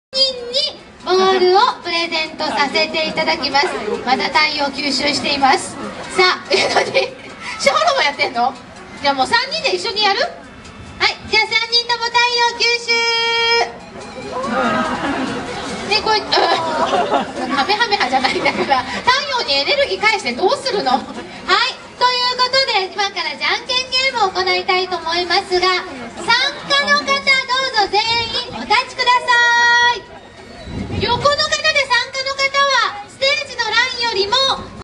みんなで光合成やったあと かめはめ波とどどん波を撃ってみたドアラ
１２月２９日から５日間続いたアピタのドアラ　シャオロン　パオロンのショーも今回が最終日

岐阜県美濃加茂市のアピタ美濃加茂店のマスコットショーを収録しています